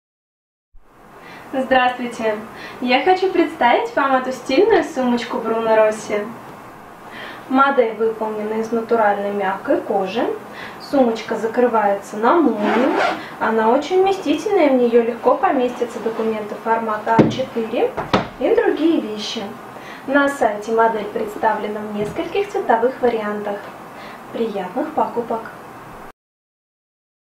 Сумка Bruno Rossi X13G brown
Любите универсальные качественные вещи? Тогда эта сумочка для вас. Стильный лаконичный дизайн, безупречность исполнения - вот главные ее достоинства. Модель выполнена из невероятно мягкой на ощупь натуральной кожи. Такой вместительный аксессуар без сомнения станет фаворитом вашего повседневного гардероба. С ним будет одинаково удобно пойти на работу или отправиться в магазин. В сумке уместятся косметика, личные вещи, книга, зонт, кошелек и масса вещей, готовых пригодиться в течение дня. Закрывается на молнию, носится на плече.
Внутри: один отдел, на задней стенке карман для документов на молнии, на передней два кармана для мелочей.
Снаружи: два вертикальных кармана на молнии на правой боковой части.